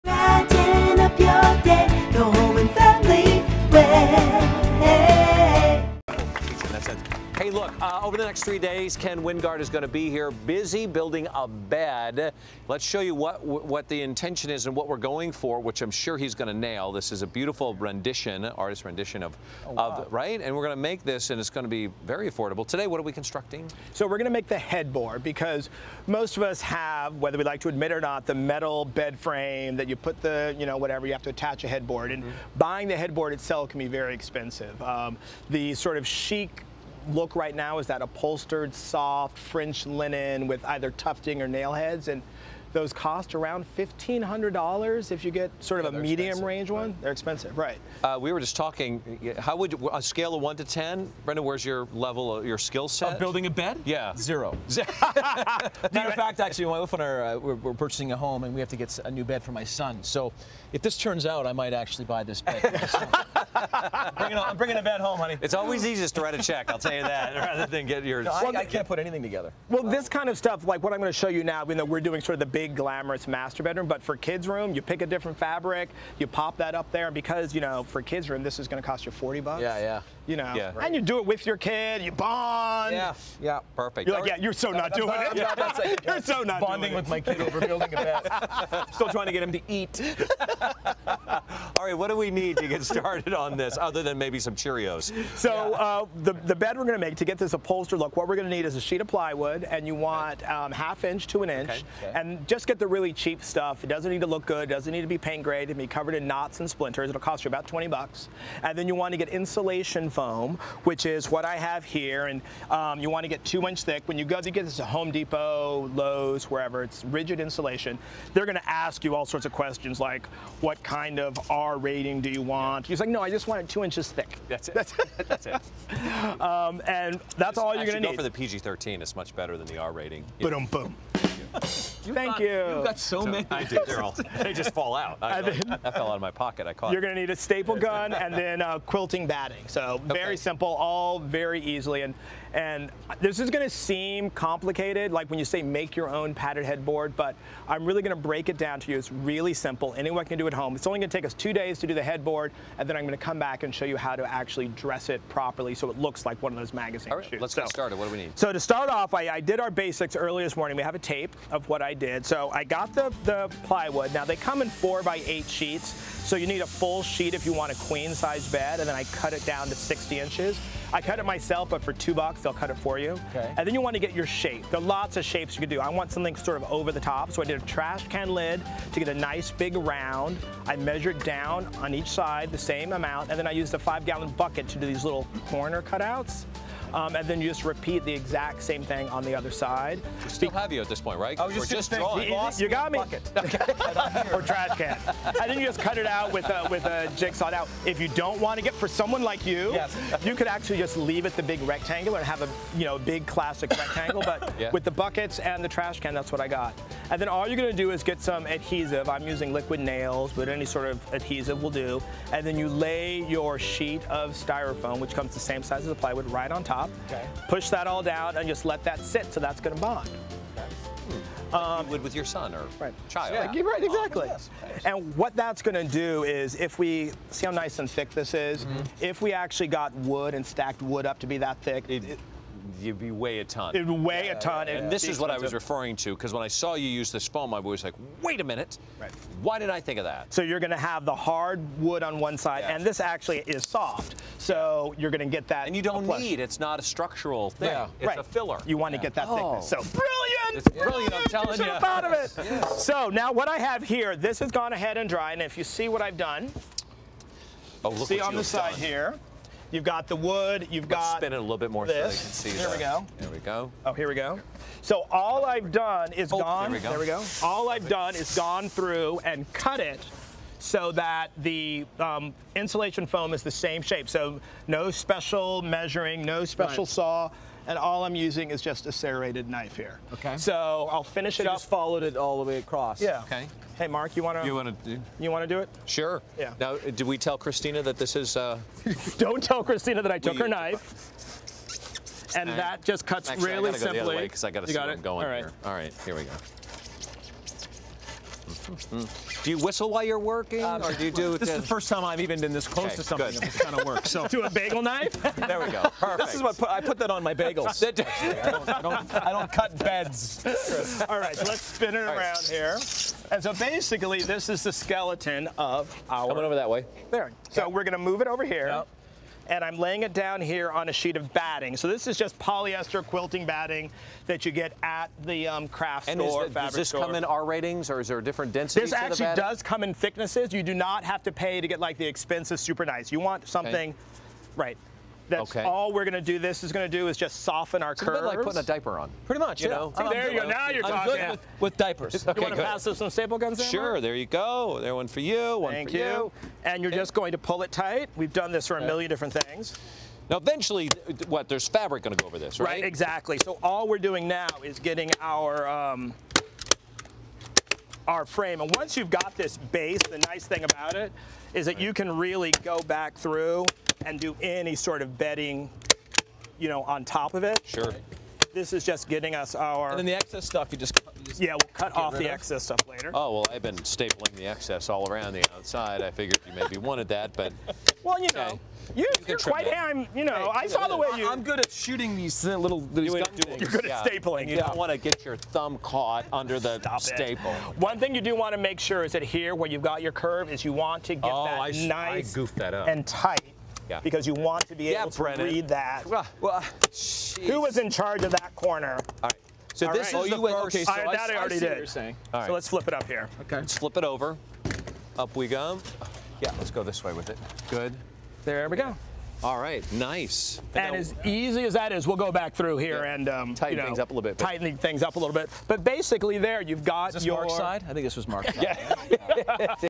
Ken Wingard's DIY Headboard Pt. 1
Lifestyle expert Ken Wingard shares a 3-part instructional piece on creating your own headboard.